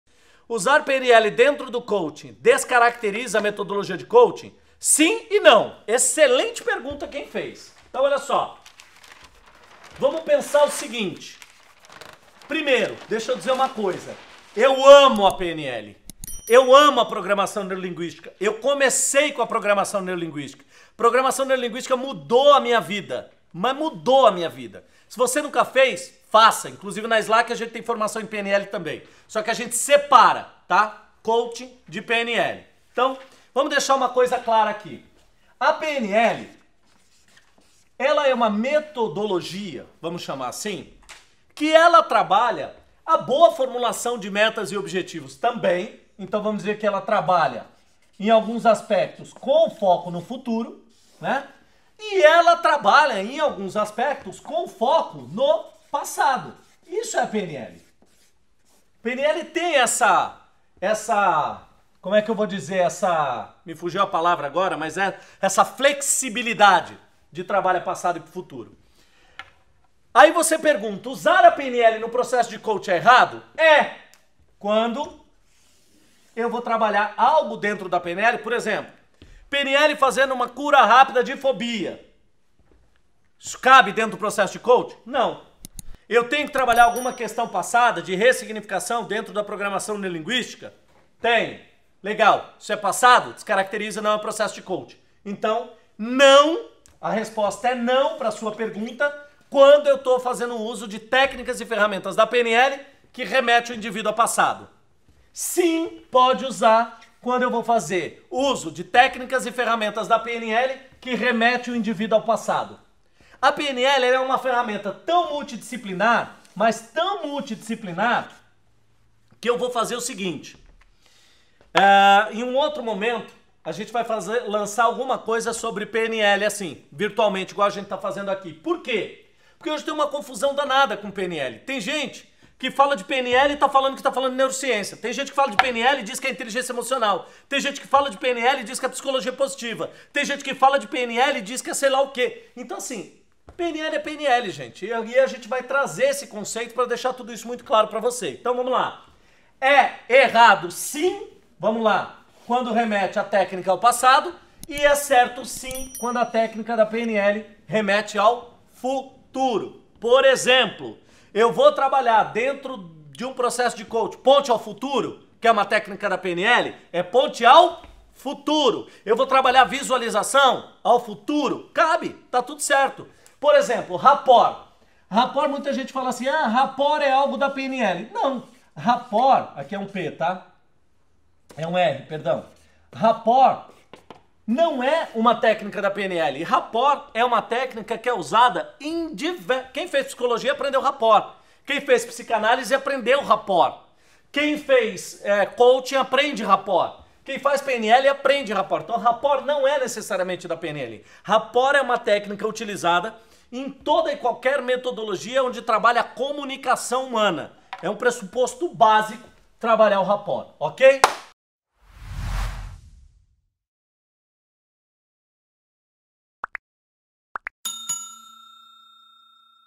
COACHING + PNL, É POSSÍVEL? | Sulivan França
Usar PNL dentro do processo de Coaching descaracteriza a metodologia Coaching? SIM e NÃO!

Saiba mais assistindo ao vídeo!